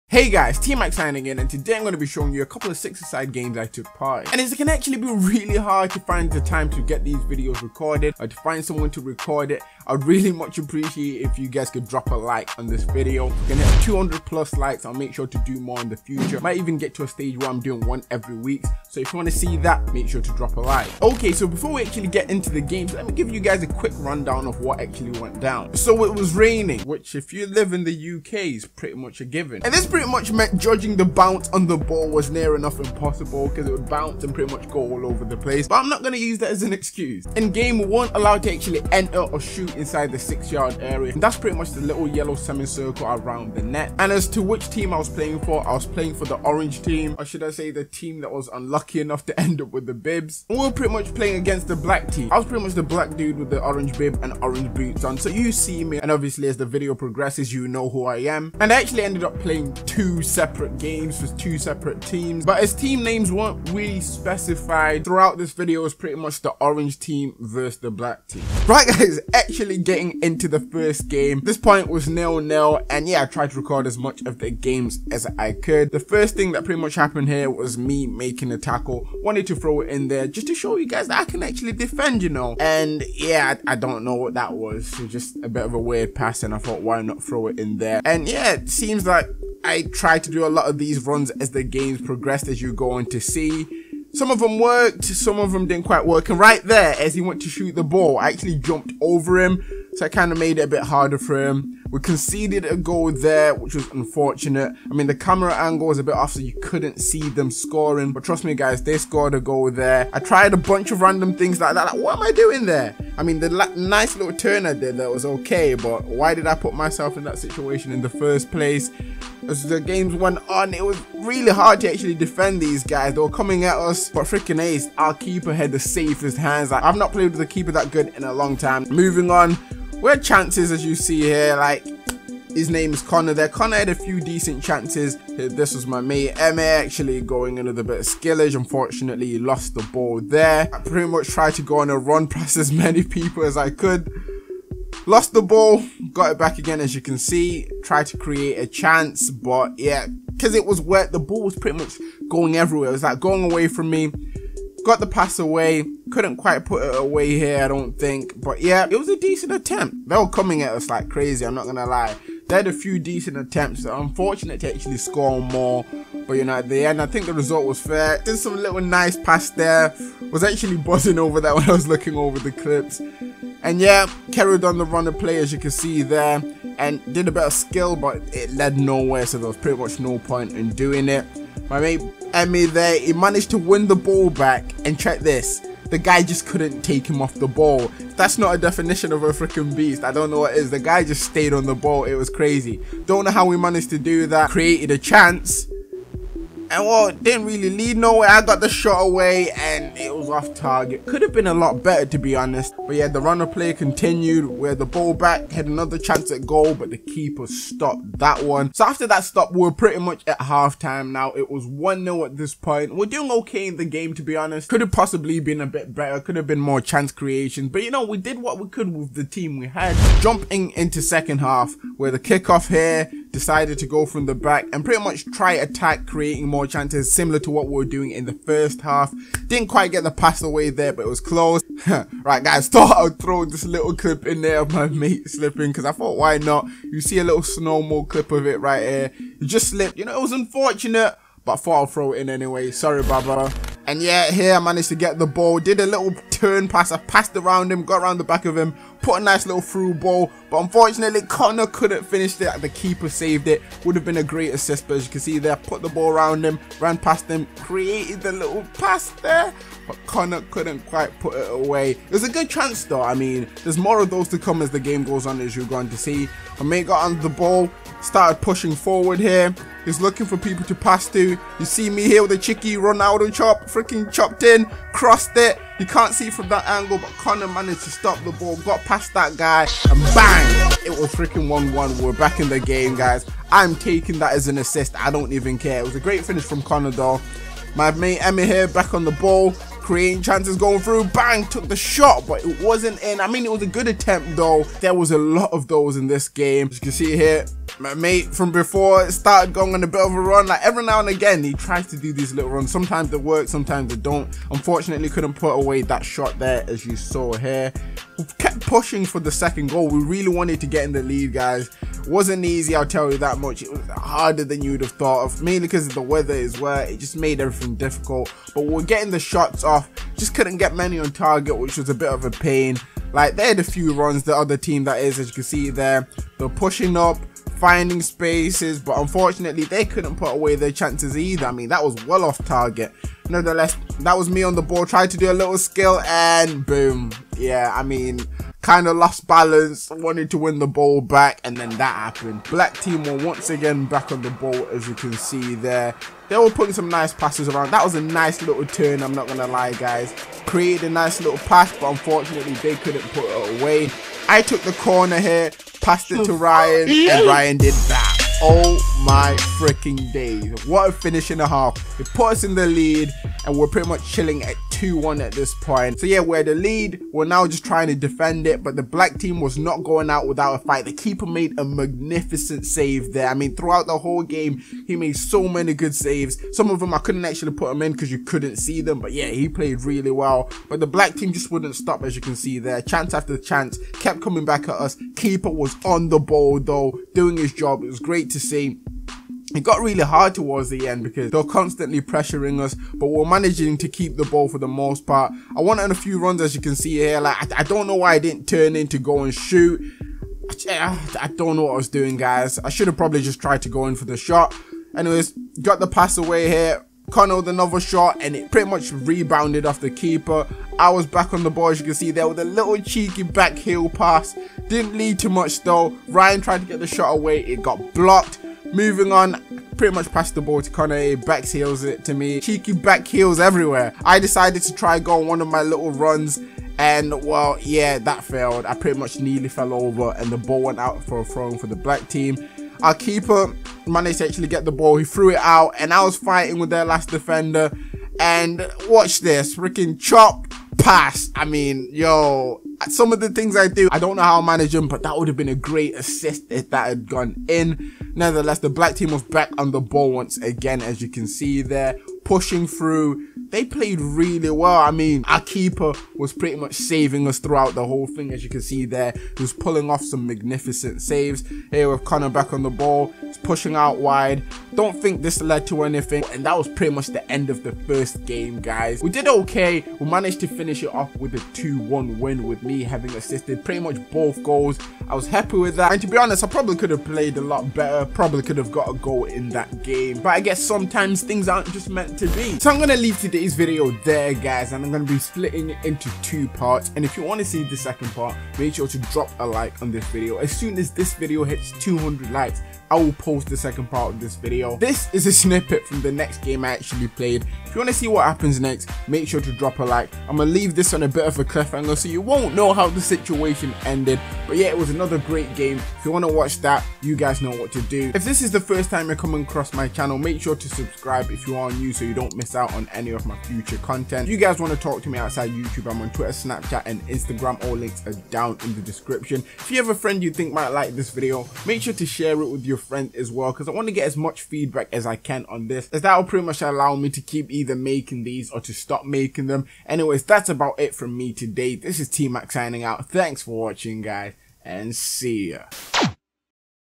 SUNDAY LEAGUE "SIX-A-SIDE" FOOTBALL! - 6 A SIDE FOOTBALL EXPIRIENCE!!
Not quite 5 a side, but here's one of my sunday league six a side games, hope you enjoy and remember to drop a like for part 2!